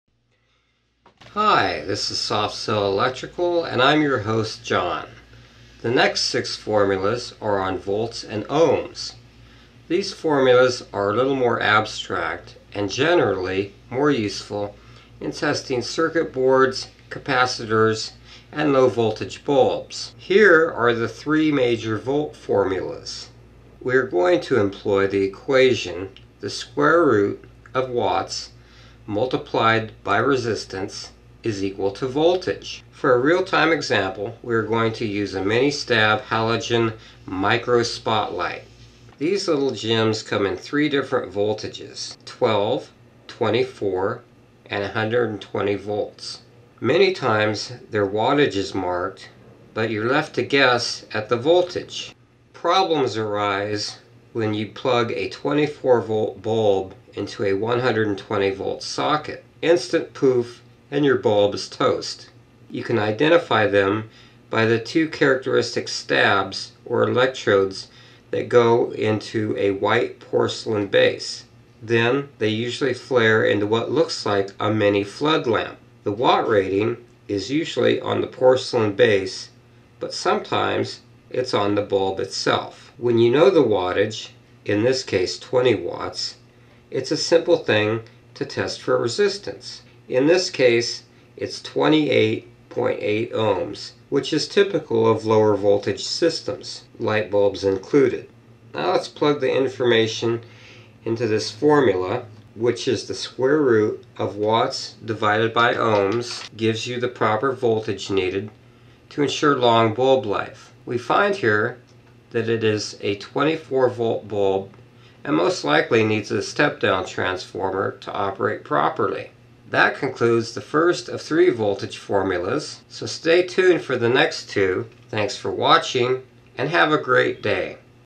testing voltage for mini bulbs # 60
# 60 This video will allow you to determine the voltage of a hard to get to bulb socket by just testing the ohms on the light conductors.  Halogens are expensive...a little less than a tester that will tell you what bulb to use when the printing is unreadable.